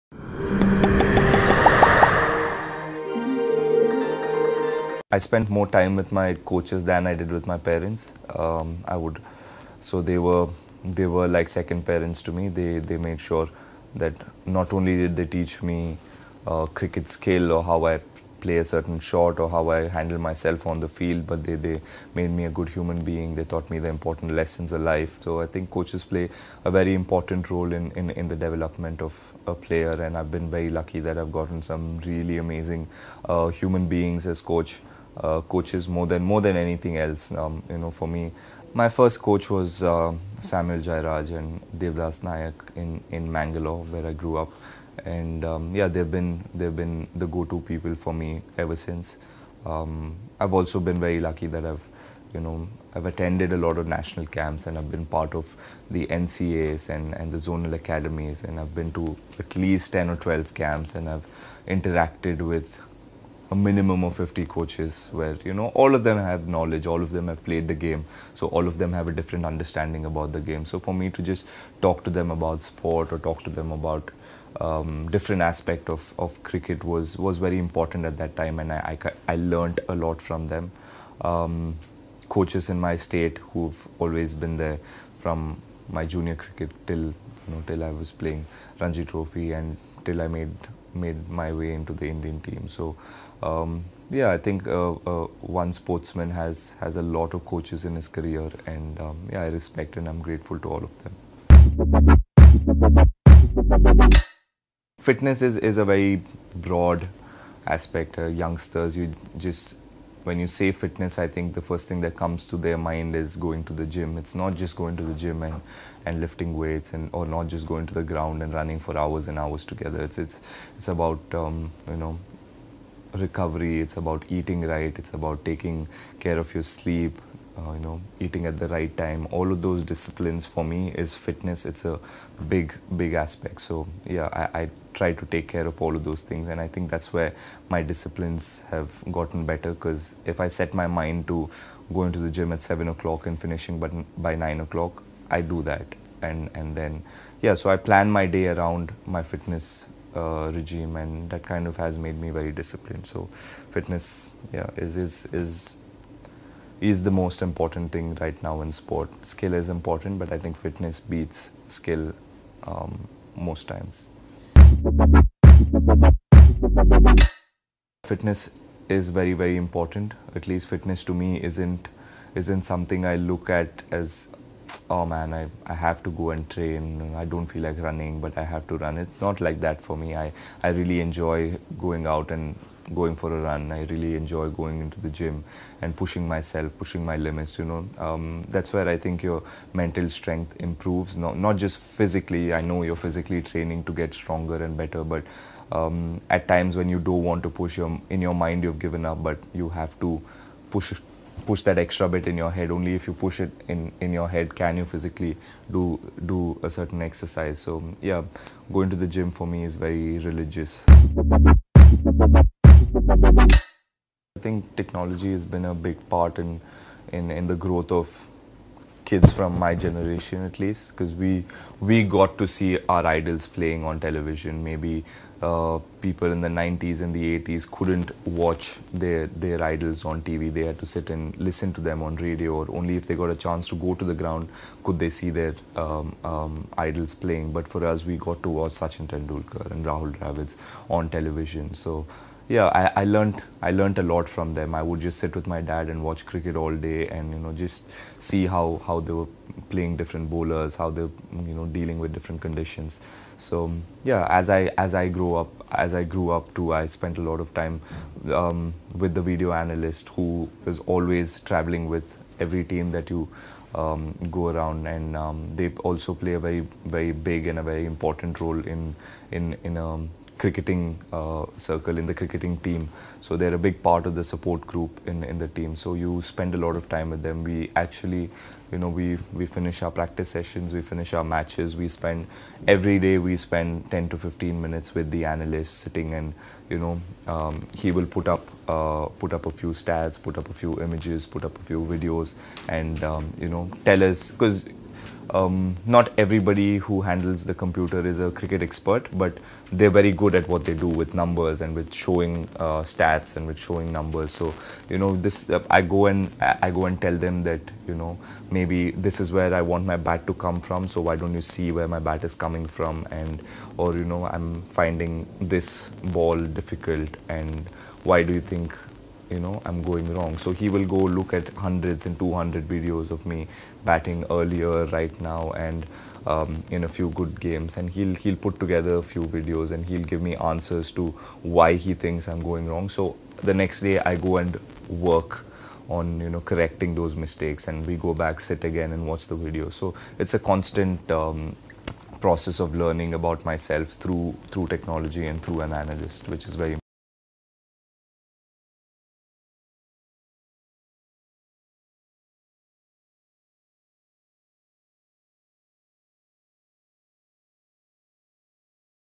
ENGLISH SPEECH | KL RAHUL,Secret to success Right Coaches ,Fitness| Millionaire English Speeches
KL Rahul's secret to success Right Coaches, Fitness, Technology | Millionaire English Speeches

*: There is no negative impact of this video on the original creation.